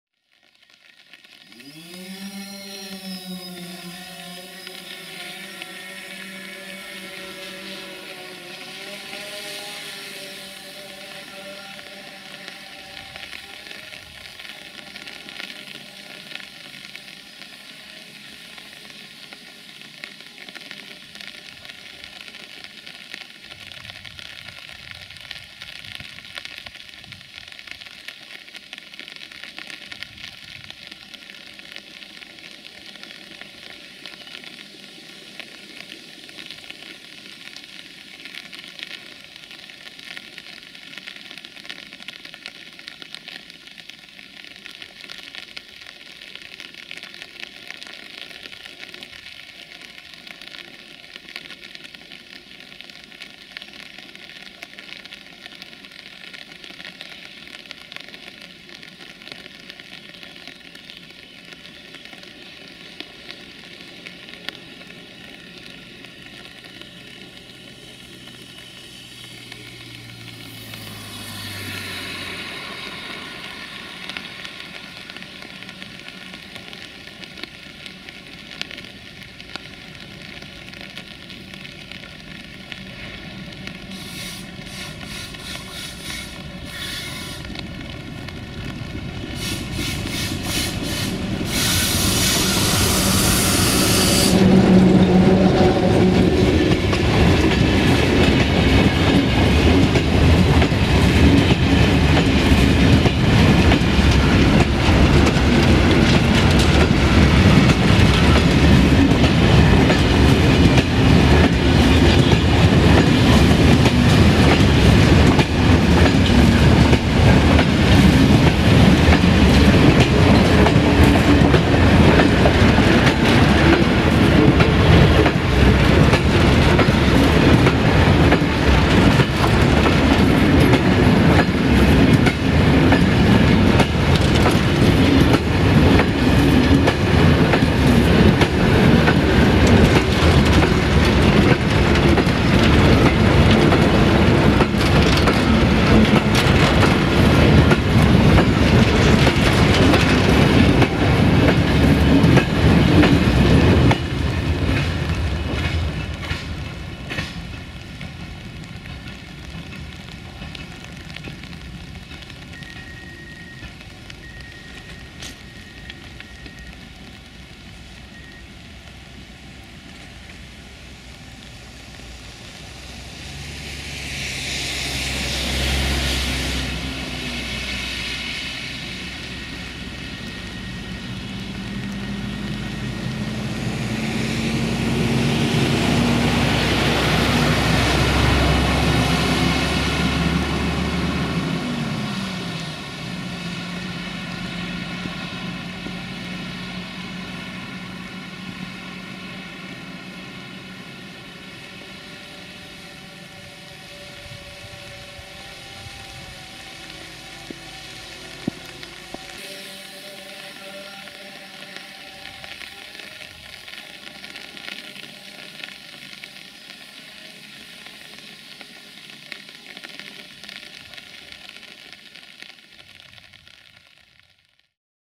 🚁[Drone] LDE2100 60-0897-3 & Marfar CFR MARFA Freight Train în Defileul Crișului Negru Canyon
🚁[Drone] LDE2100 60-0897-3 & Marfar CFR MARFA Freight Train în Defileul Crișului Negru Canyon - 07 July 2020

1:CFR MARFA Freight Train/Marfar/Guterzug - Fluieroasa/Whistle Loko 060-DA/LDE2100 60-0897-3 RO-SNTFM

07 July 2020